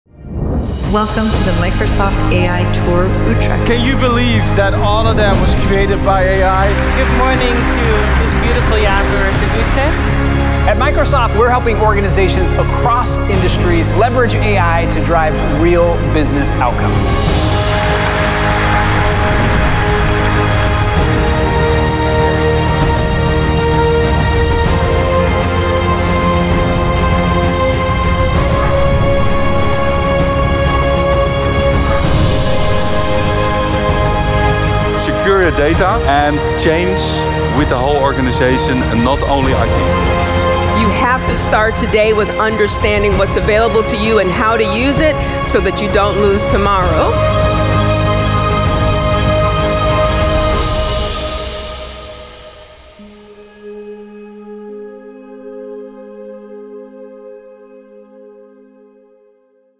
Microsoft AI Tour Utrecht 2025 Recap
Wow! What an incredible day! We’re still feeling the energy from the Microsoft AI Tour in Utrecht! ✨

From inspiring keynotes to hands-on sessions, the tour showcased how AI is shaping our future. 🎉 A huge thank you to everyone who made this event a success: attendees, speakers, featured partners and organizers. We’ve gathered some of our highlights here, but we’d love to hear from you too. What were your key takeaways or favorite moments?